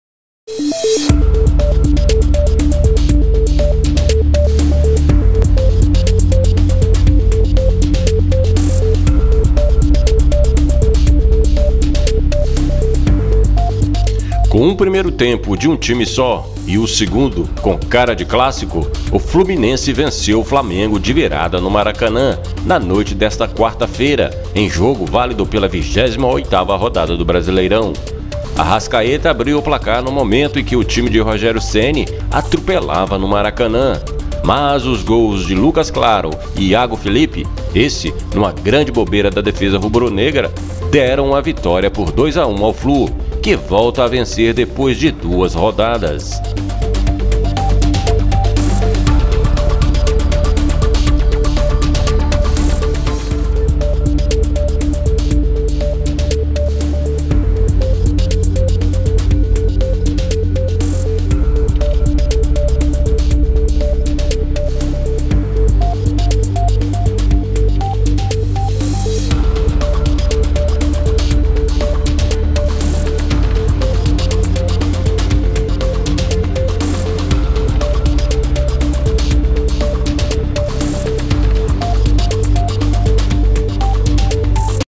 √ Fluminense castiga o Flamengo no Maracanã I 07.01.2021
Fluminense castiga o Flamengo nos acréscimos e vence o clássico de virada.Flamengo domina o primeiro tempo, mas Flu iguala as forças no segundo, aproveita bobeira da defesa rubro-negra para virar no fim e entra de vez na briga por uma vaga na Libertadores. Fla perde chance de se aproximar do líder.

Resumão
Com um primeiro tempo de um time só e o segundo, sim, com cara de clássico, o Fluminense venceu o Flamengo de virada no Maracanã na noite desta quarta-feira, em jogo válido pela 28ª rodada do Brasileirão. Arrascaeta abriu o placar no momento em que o time de Rogério Ceni atropelava no Maracanã, mas os gols de Luccas Claro e Yago Felipe (esse numa grande bobeira da defesa rubro-negra) deram a vitória por 2 a 1 ao Flu, que volta a vencer depois de duas rodadas.

Alô, Libertadores!
Com a vitória no clássico, o Fluminense entrou de vez na briga por uma vaga na Libertadores. Sétimo colocado, o time do técnico Marcão chegou aos 43 pontos, um a menos que o Palmeiras, que é o sexto. Nesse momento, o Flu estaria fora da Libertadores. Mas existe uma grande possibilidade de o G-6 virar G-7, caso o vencedor da Copa do Brasil (a final será entre Grêmio x Palmeiras) termina o Brasileirão entre os seis primeiros. E também há a chance de virar G-8 se o campeão da Libertadores também terminar na parte de cima da tabela - Santos e Palmeiras estão na semifinal da competição. Ou seja, o Flu está ali no bolo.

Castigo no fim
O Flamengo teve inúmeras oportunidades de gol nesta quarta-feira. Com cinco minutos de jogo, por exemplo, já haviam sido duas: Gabigol perdeu chance clara chutando por cima e Rodrigo Caio parou nas mãos de Marcos Felipe. No segundo tempo, o Fluminense foi para cima, equiparou as forças, chegou ao empate com Luccas Claro e finalizou o rival aos 47 minutos do segundo tempo, quando Arão e Hugo Souza titubearam depois do passe de Filipe Luís, e Yago Felipe apareceu entre eles tocando de mansinho para o fundo das redes. Quem não faz...

Na bronca
Na saída de campo, Arrascaeta, autor do gol do Flamengo, deu entrevista forte criticando bastante a equipe. "A gente tem que sair com vergonha hoje porque o jogo a gente está perdendo. Tem hora que a gente não merece ser campeão", disse o uruguaio.

Perdeu a chance...
O Flamengo desperdiçou uma grande oportunidade de se aproximar do líder São Paulo, que perdeu para o Bragantino na rodada. Dessa forma, portanto, a diferença segue de sete pontos entre eles: o Flamengo é o terceiro, com 49, enquanto os paulistas têm 56. Já o Fluminense é o sétimo colocado, com 43 pontos.

Flamengo 1 X 2 Fluminense,
Flamengo 1 X 2 Fluminense Brasileirão 2020,
Flamengo 1 X 2 Fluminense Melhores Momentos,